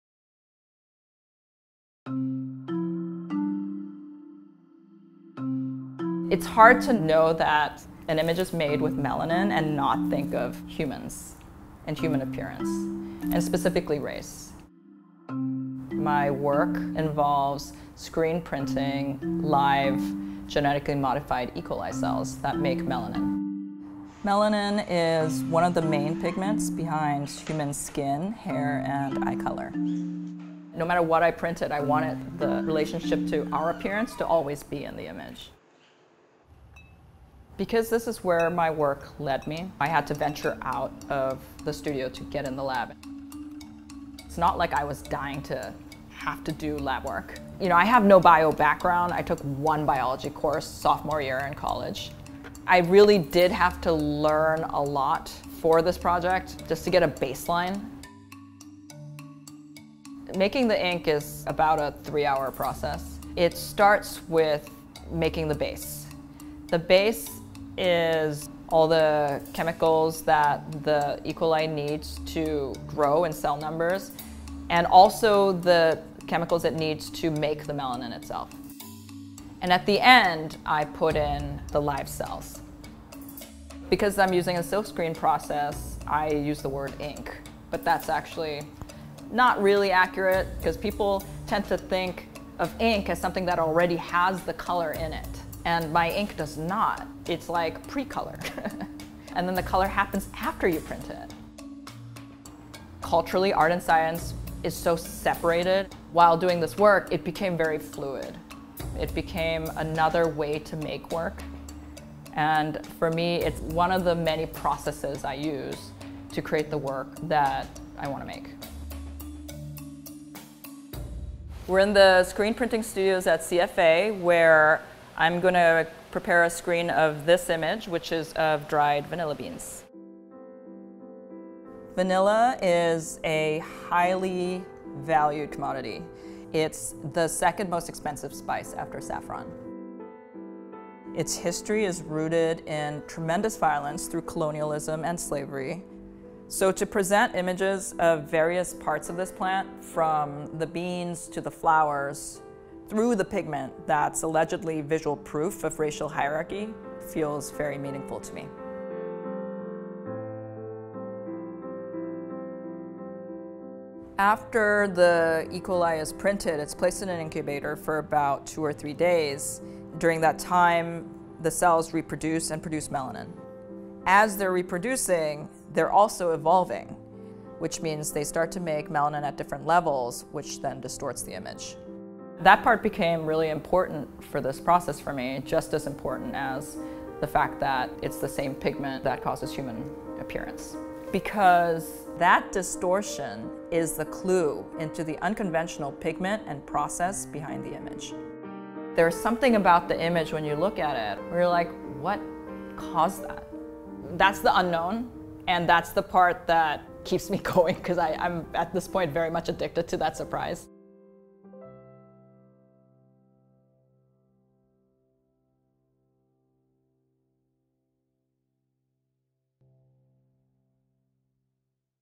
CFA Professor Lucy Kim Creates Images Using Genetically Modified E.coli That Produce Melanin
What do melanin and E.coli have in common? Well, Lucy Kim, an award-winning visual artist, and BU college of fine arts associate professor uses melanin to create captivating monochromatic screen prints, pushing the boundaries of art and science. The melanin is a product of genetically modified E.coli.